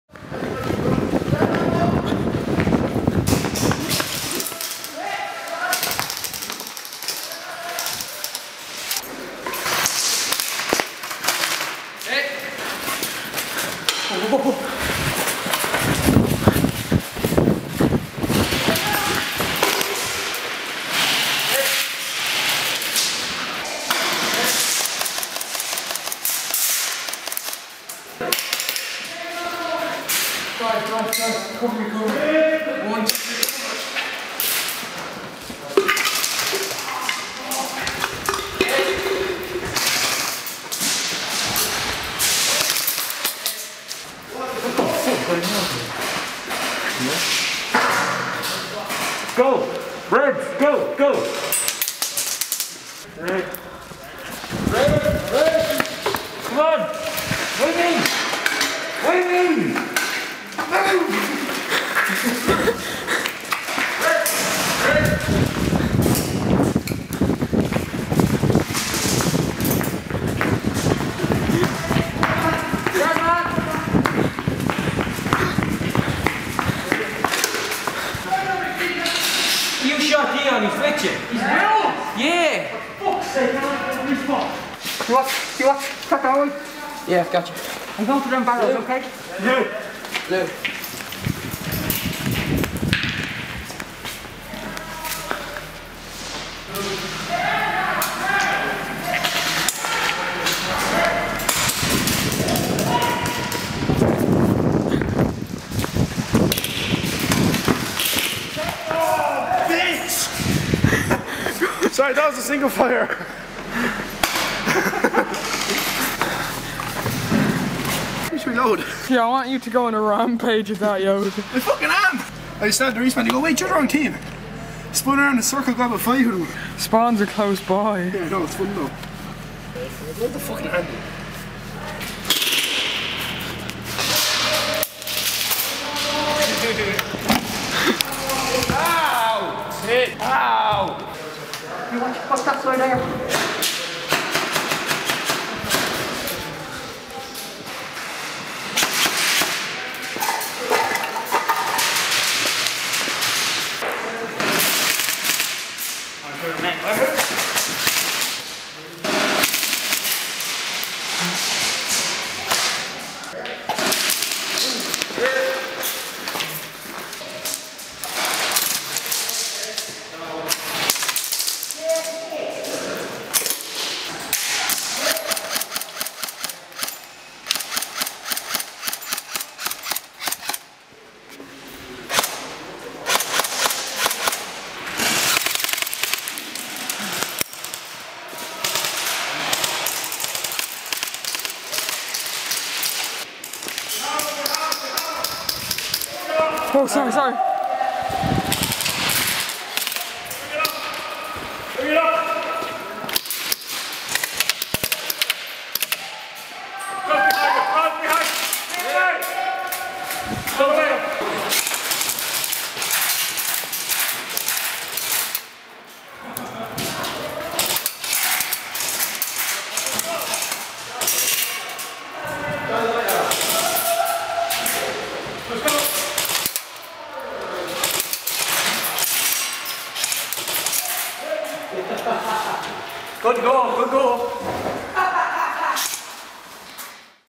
Epic Airsoft Action HD - Bomb Game - Vacant Airsoft Ireland
This was filmed at vacant airsoft.
Vacant airsoft is located in naas Co.kildare,Ireland. exit 9 off the naas dual carriage way. opposite storm cinemas.
"Irelands best cqb airsoft site". "biggest cqb airsoft site in europe with over 2.5 million square feet of madness at your disposal"

Thanks to the Marshals for letting us film.

Filmed with a canon 550d

this was filmed on the 11/12/11
Best irish cqb site